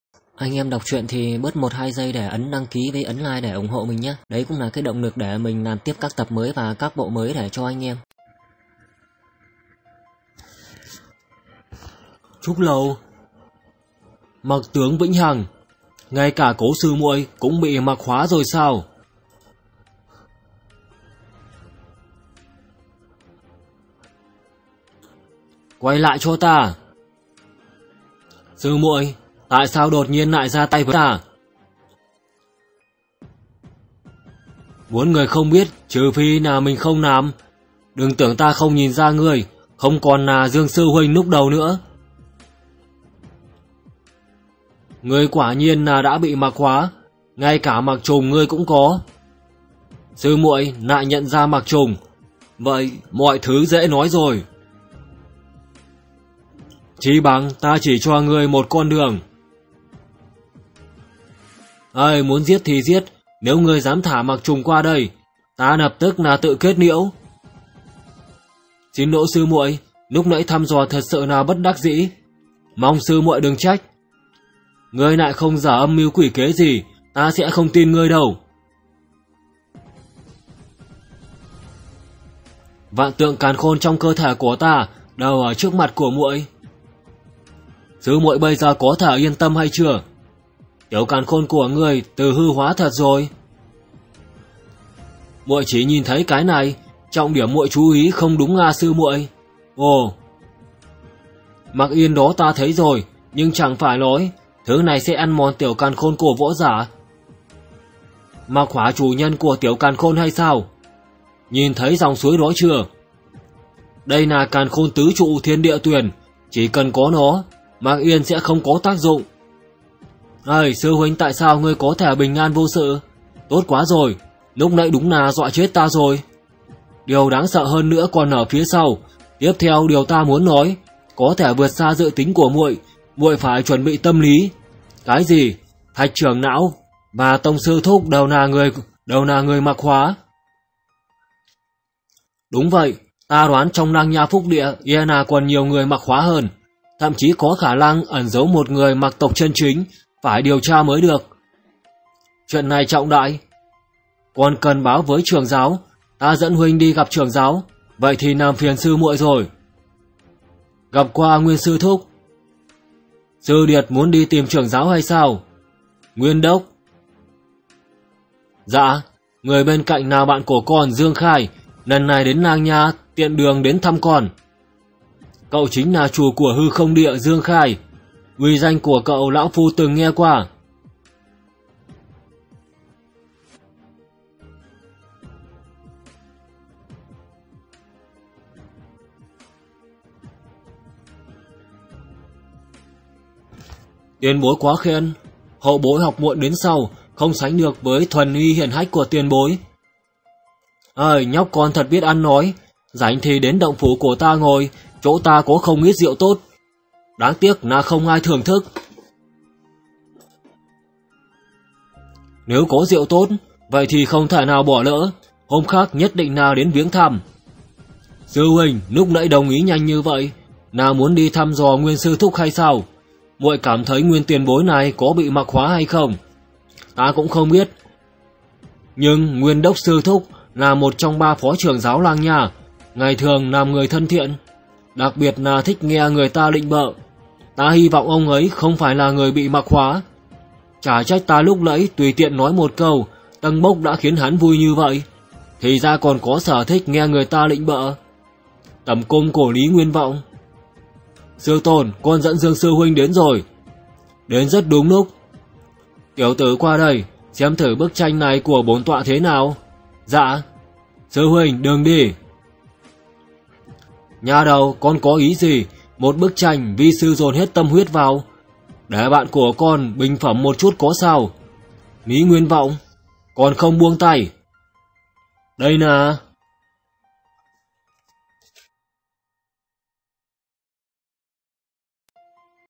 Võ Luyện Đỉnh Phong Tập 2944|Dương Khai thiếu niên phế vật nỗ lực đi lên đỉnh cao của Võ Đạo
►►►►►► Nguồn Truyện: Vcomi.me  - Hamtruyenmoi.com - Manhuarock.net - Toptruyen.net - Truyensieuhay.com  - Vcomic.net  - Truyentranhaudio.com